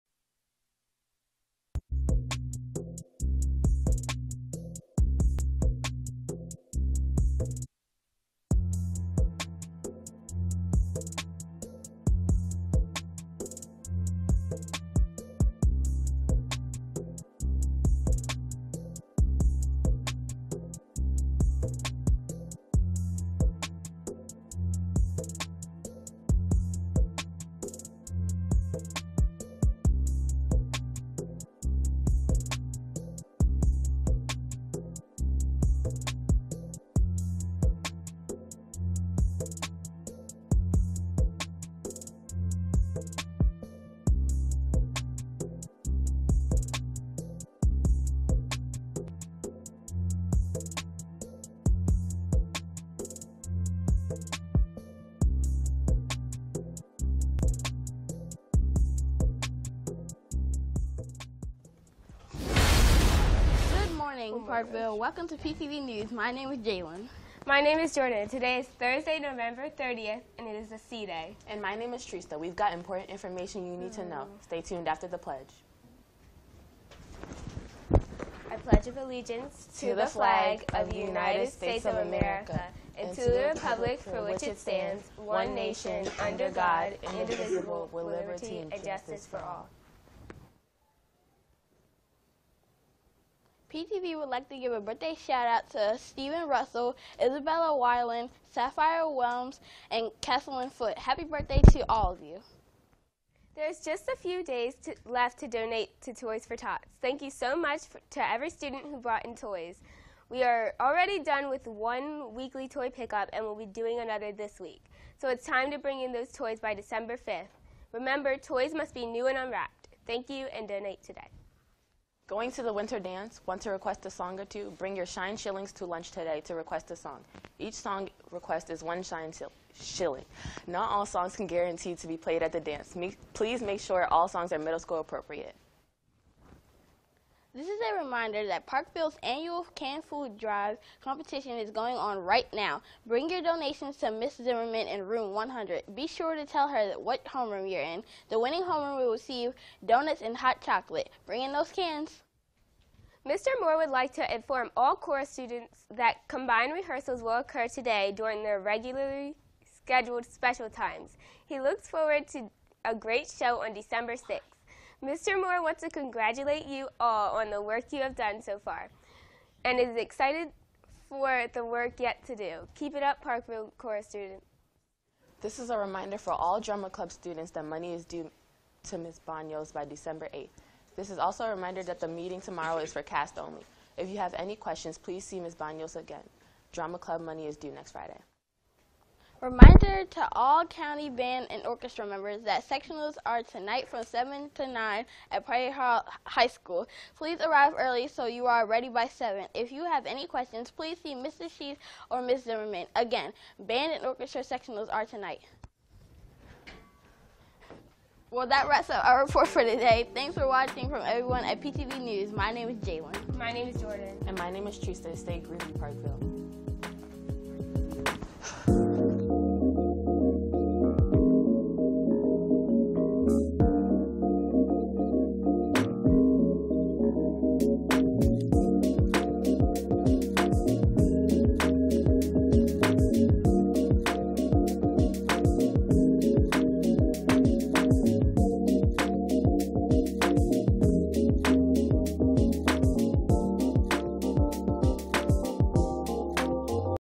PTV News -- November 30, 2017
PTV News from November 30, 2017

PTV News is a daily show streamed live every school day at Parkville Middle School.

There are no copyright infringements intended for the use of the song(s) or picture(s) within this video.